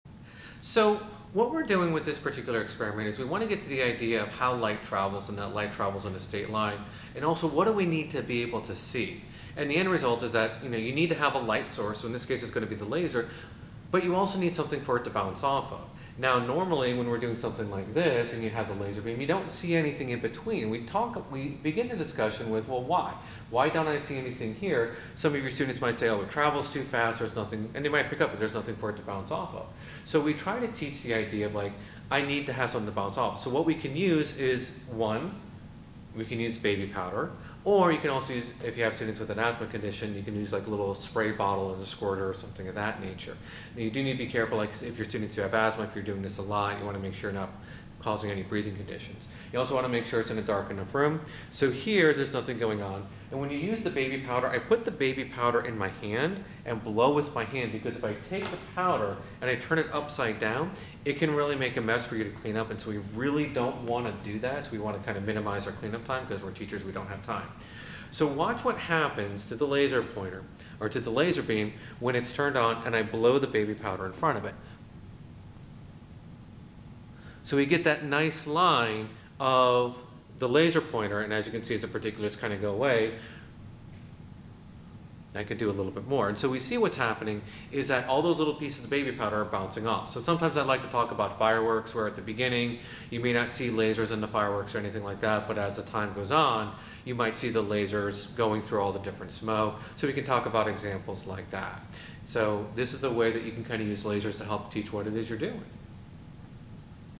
Teacher discussion for light traveling in a straight line
In this demo, I talk to teachers about to teach the activity in their class.